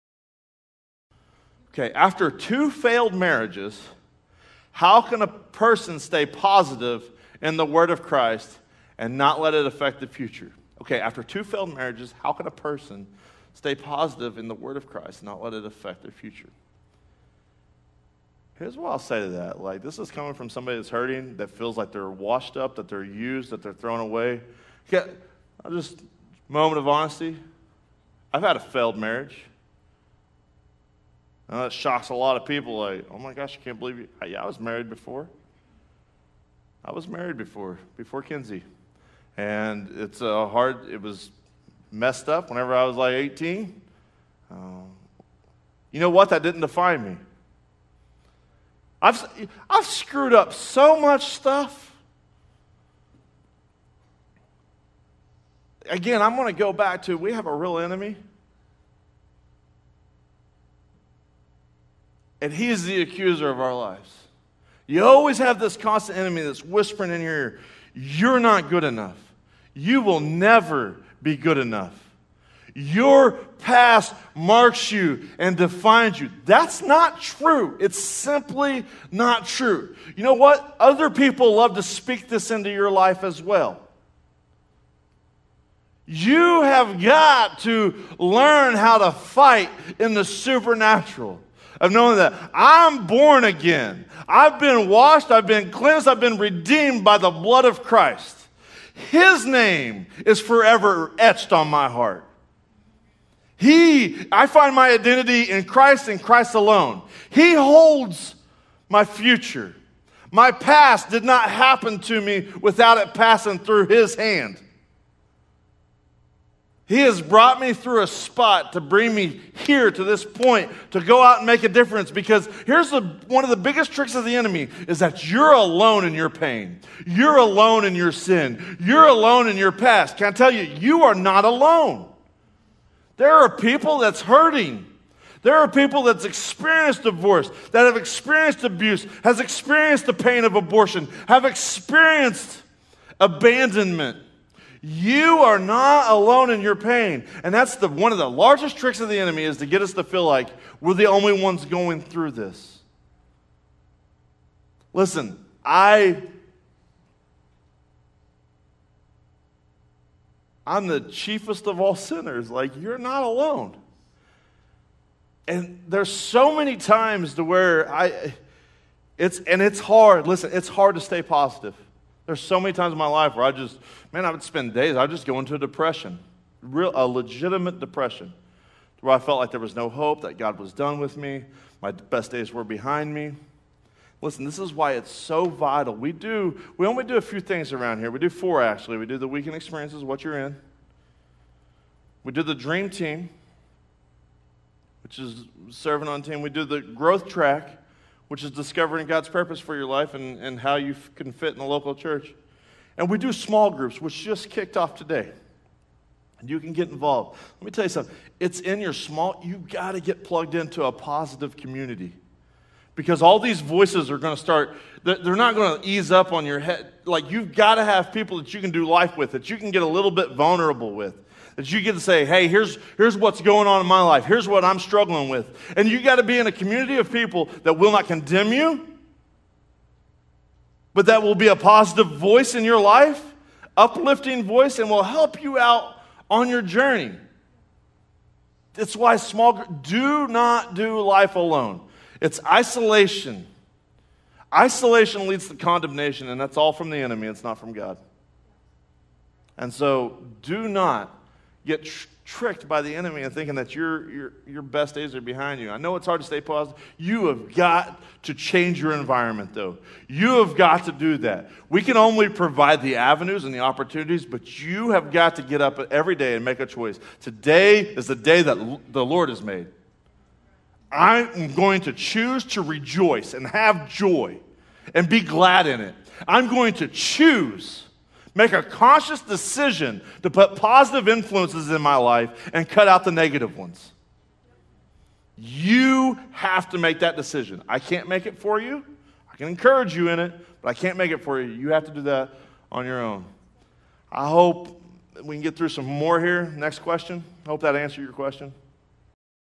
After 2 failed marriages, how can a person stay positive in the word of Christ? - 1st Experience
If you have made a decision for Christ or responded to this message in any way we would love to hear from you.